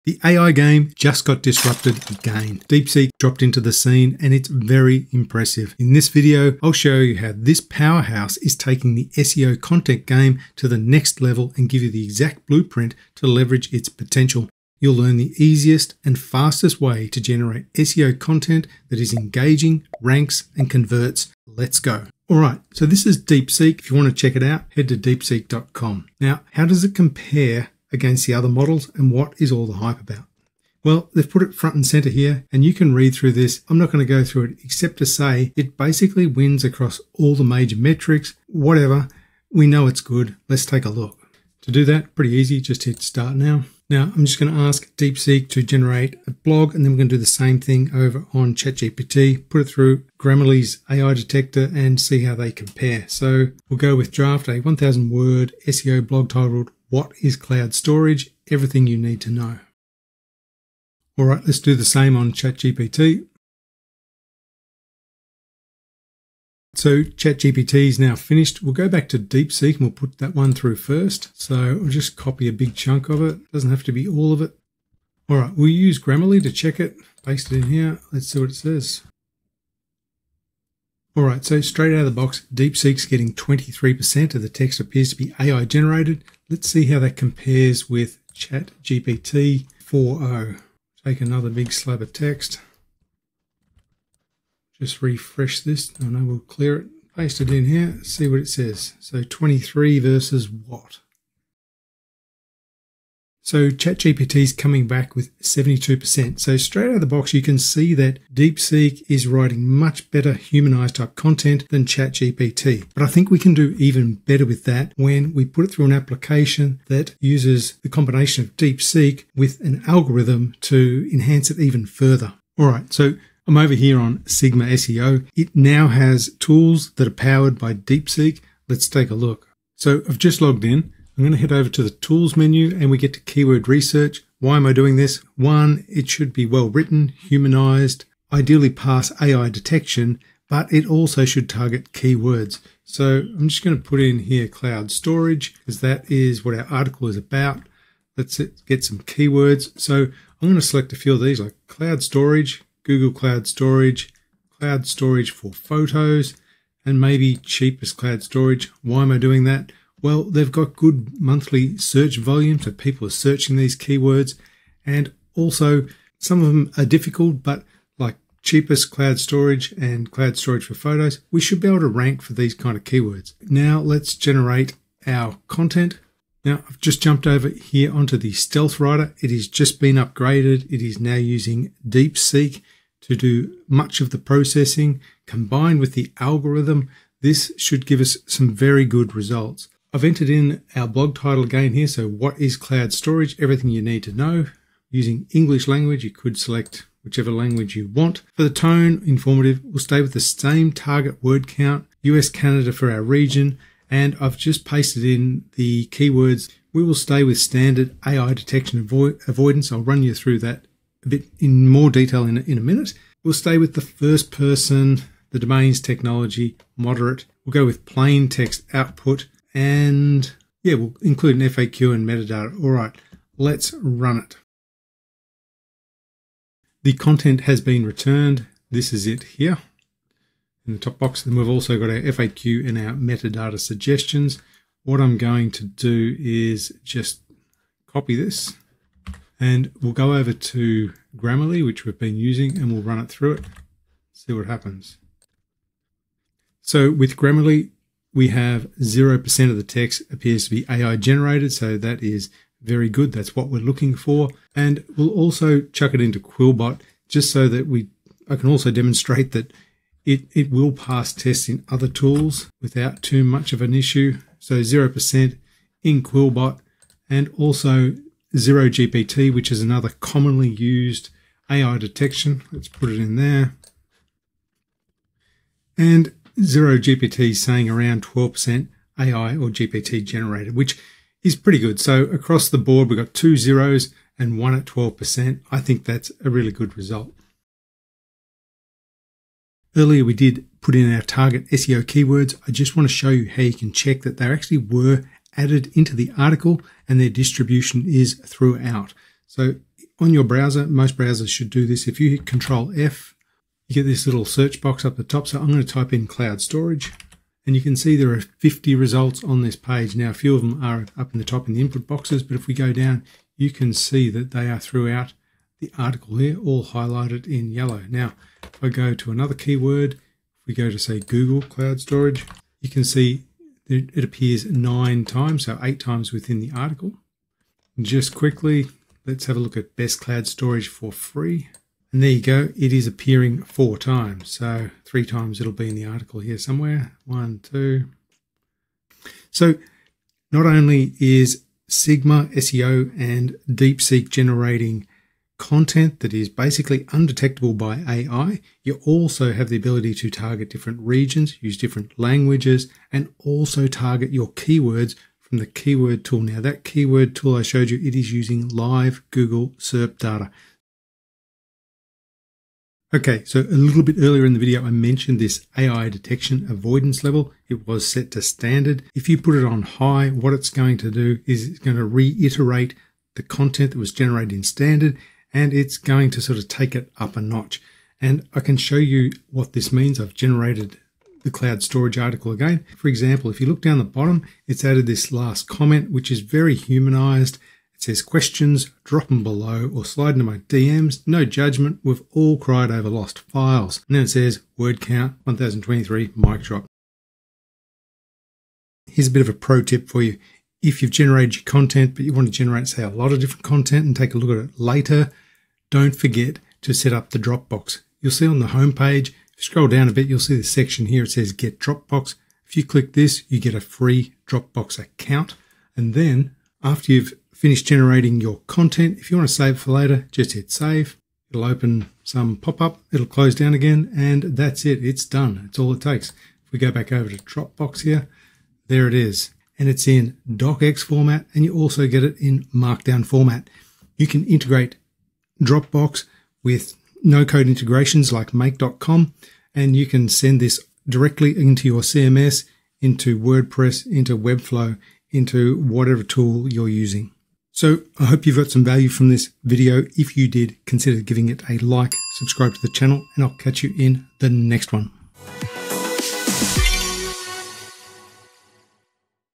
AI SEO Content ➡️ DEEPSEEK R1 vs ChatGPT (Guess The Winner 🥇)
Want to stay ahead of Google's AI detection game?

The DEEPSEEK R1 is here to help! In this video, we'll explore the cutting-edge features of this innovative tool and how it can help you outsmart Google's AI detection. From cleverly crafted content to sophisticated algorithmic evasion, we'll dive into the world of AI-powered SEO and uncover the secrets to dominating search engine rankings.

Whether you're a seasoned SEO expert or just starting out, this video is a must-watch for anyone looking to stay one step ahead of the competition. So, what are you waiting for?

Click play and discover the power of DEEPSEEK R1 for yourself!